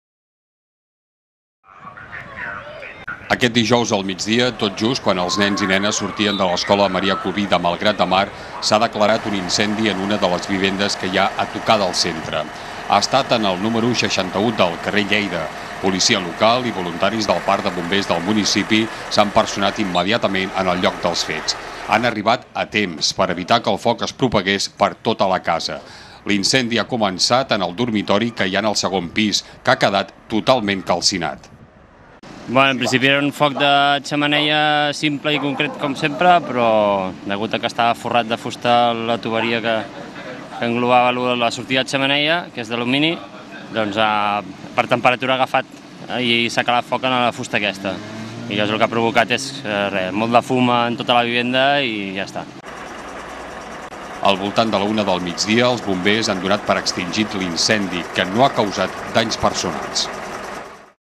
compte amb les xemeneies
vídeo informatiu sobre l'incendi en una vivenda al carrer Lleida de Malgrat de Mar